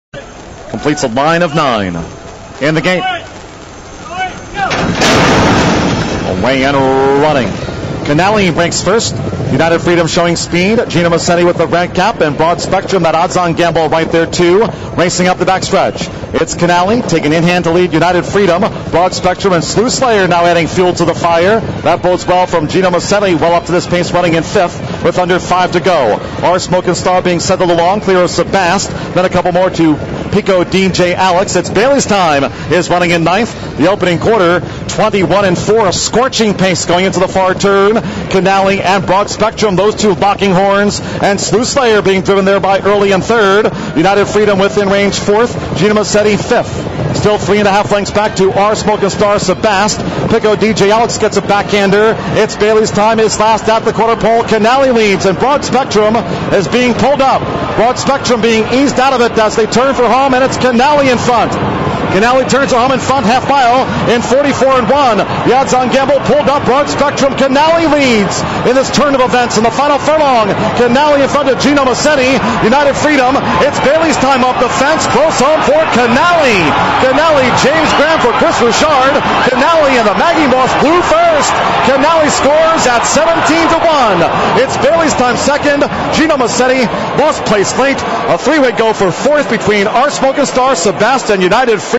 ARLINGTON PARK, 2007-08-09, Race 6
Entries: Thoroughbred Race, ARLINGTON PARK, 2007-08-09, Race 6:
      (Pgm-Runner-Jockey-Finish)
       1-Pico D J Alex (IL), Lantz, J, finished 7;
       4-Its Bailey's Time (KY), Lopez, U, finished 2;
       5-Sebast (KY), Meza, N, finished 6;
       6-Broad Spectrum (KY), Baird, E, finished 9;
       8-Slew Slayer (NY), Fires, E, finished 8;
       9-Canali (KY), Graham, J, finished 1;
      10-R Smoken Star (KY), John, K, finished 4;
      11-Gino Massetti (IL), Sanchez, D, finished 3;
      12-United Freedom (IL), Sterling, Jr., L, finished 5;